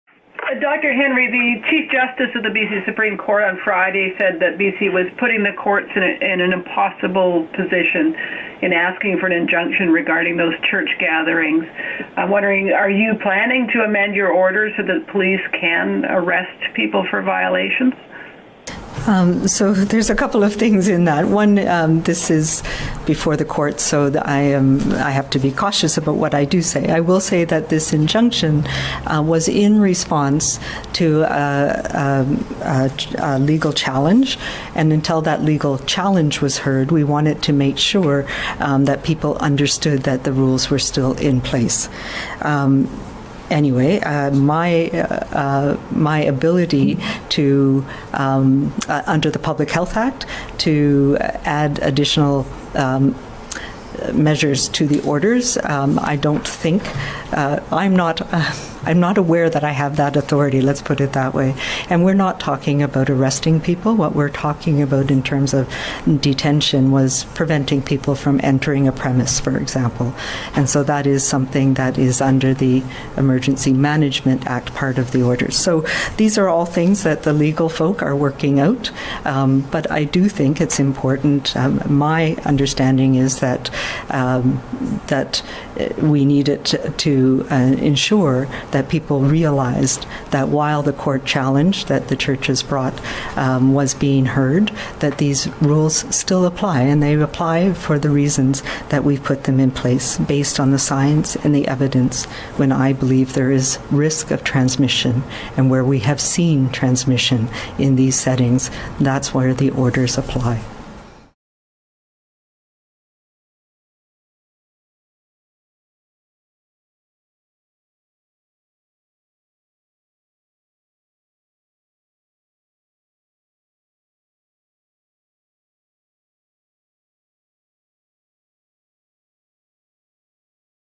COVID-19: Injunction to let church challengers know virus rules still in place | Vancouver Sun
Dr. Bonnie Henry gives her regular coronavirus update on Feb. 16, 2021.